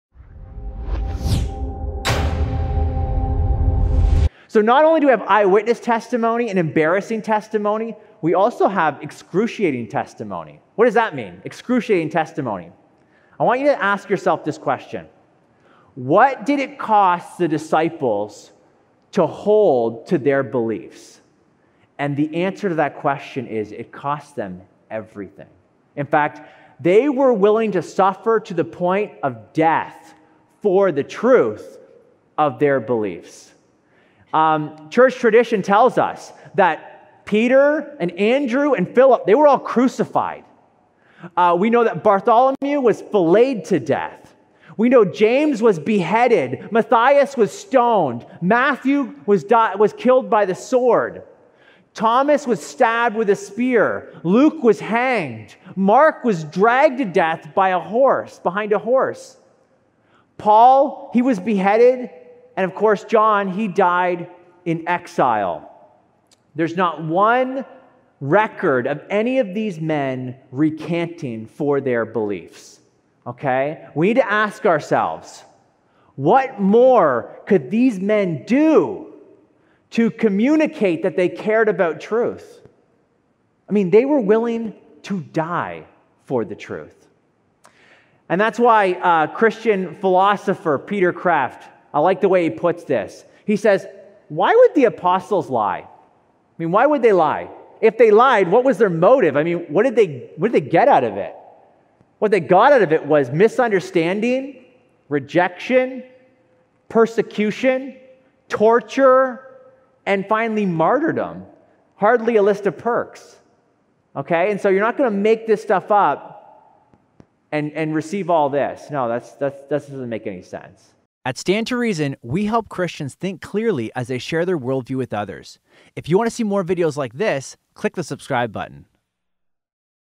They Gave Their Lives for the Truth
Tim Barnett of @RedPenLogic explains how the disciples had nothing to gain by making up what they professed about Jesus.

CONNECT

Have a question or comment? If you'd like to submit your question ahead of time, fill out the online form

GIVE